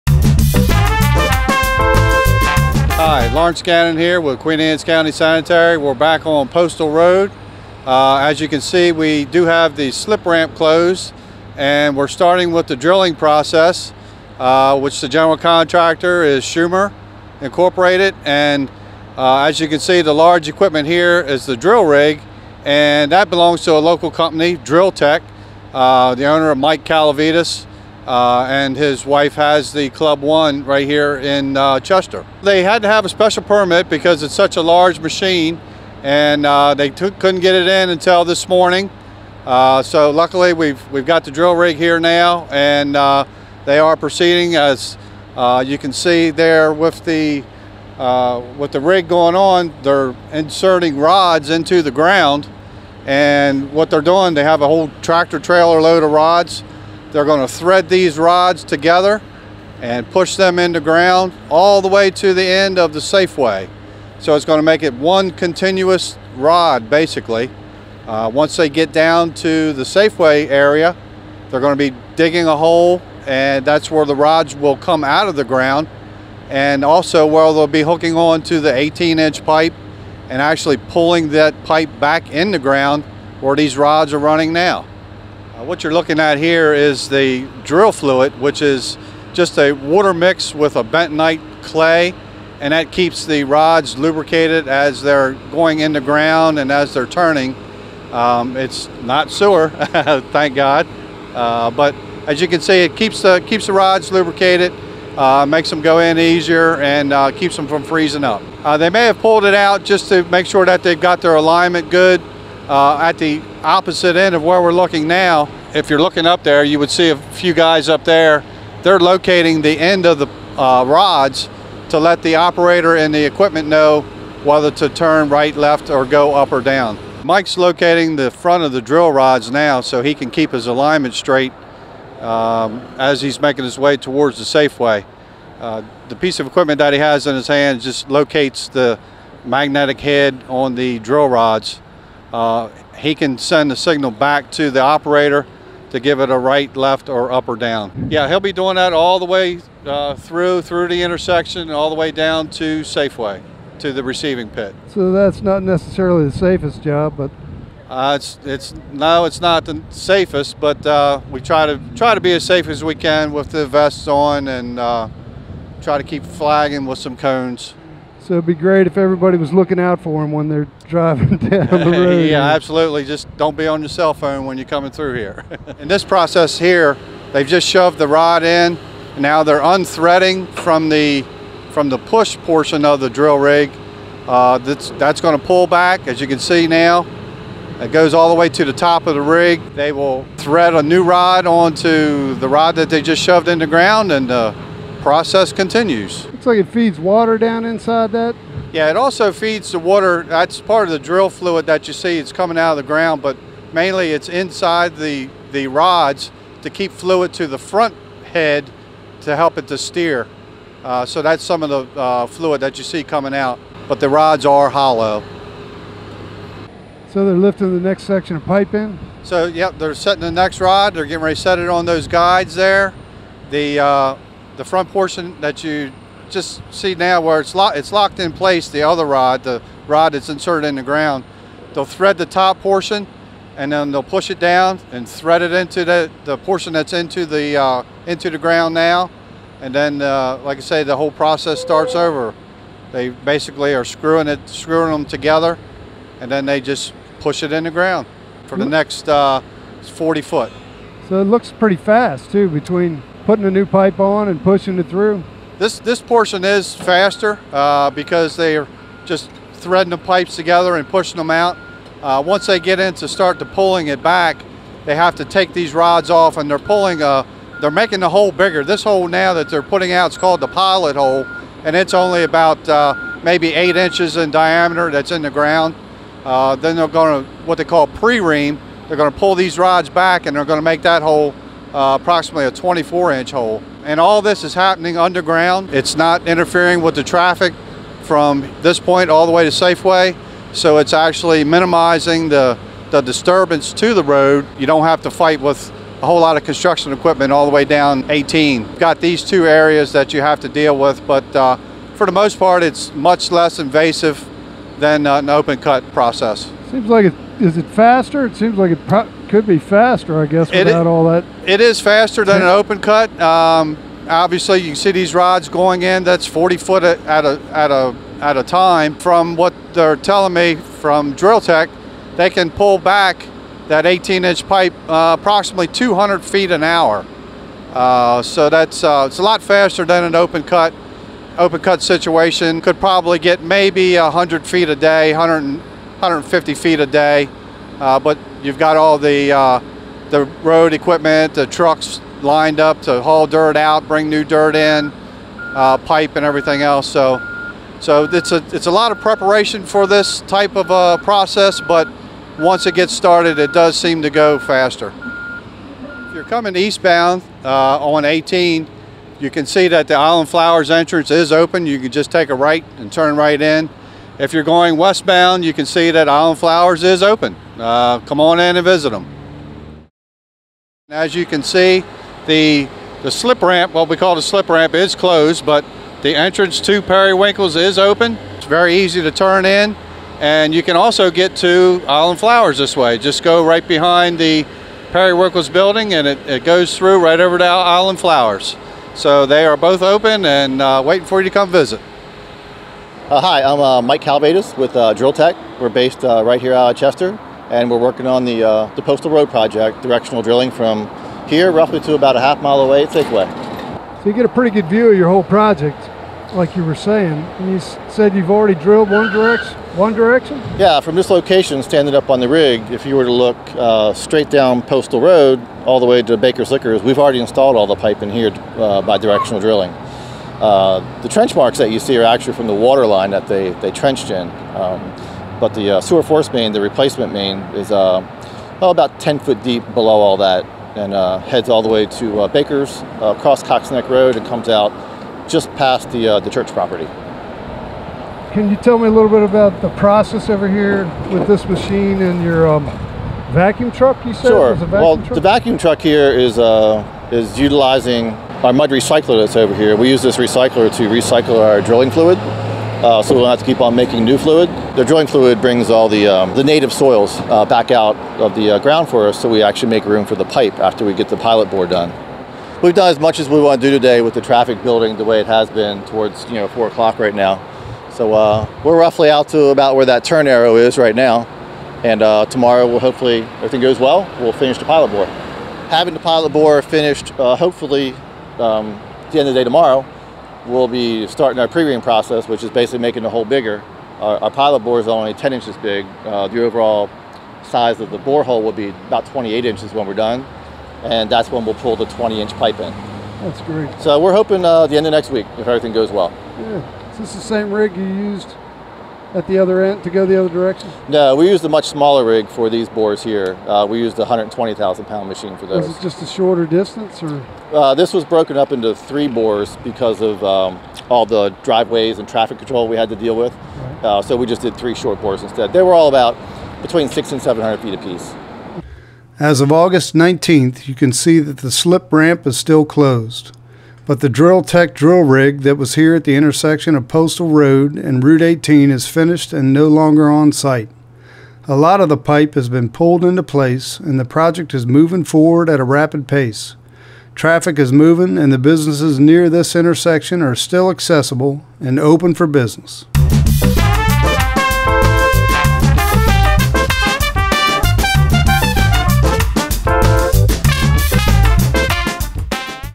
POSTAL ROAD CONSTRUCTION UPDATE AUGUST 22, 2014 1
Update on the Postal Road construction project since the end of July, 2014. Provides some explanation of the process, how the big drill rig worked and additional details.